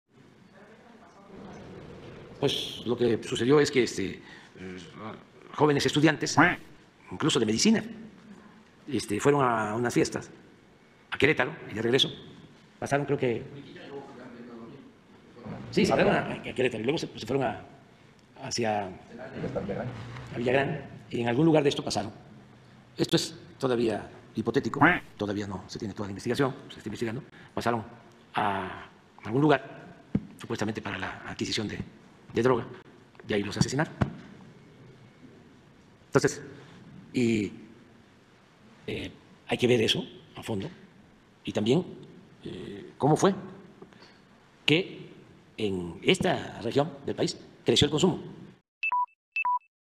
El Pdte. López Obrador revictimiza a los 5 estudiantes 4sesinados y torturados en Celaya, Gto. 🫤
El Pdte. López Obrador, revictimiza a los 5 estudiantes de medicina asesinados y torturados en Celaya